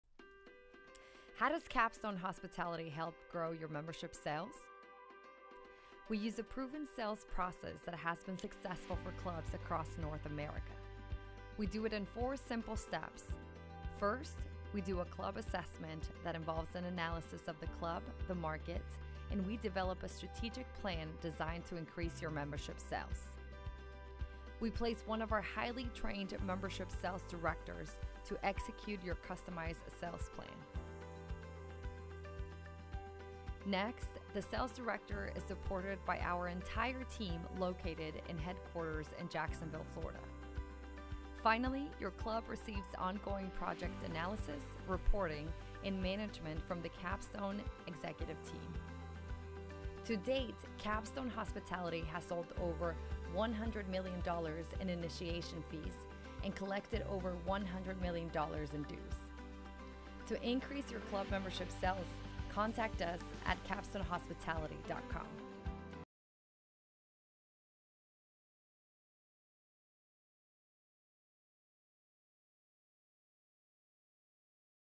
Increase Your Club's Membership Sales
Are you looking for new ways to increase membership sales for your club? Look no further! In this video, we'll show you how Capstone Hospitality increases your club's membership sales. Our proven methods includes recruitment, training, placement, management, & most importantly, sales. There's no better time to start growing your club's membership - so watch this video and get started today!